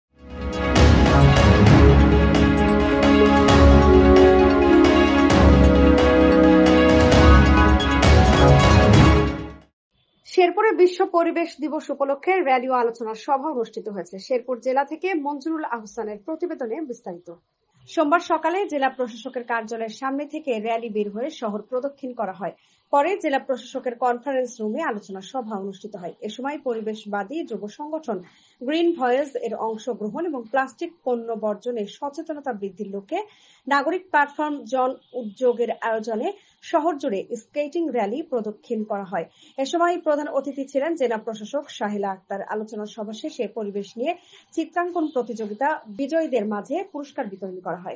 শেরপুরে বিশ্ব পরিবেশ দিবস উপলক্ষে র‌্যালী ও আলোচনাসভা অনুষ্ঠিত হয়
*** ANTI-PIRACY WARNING ***

bangla Newsw
BD News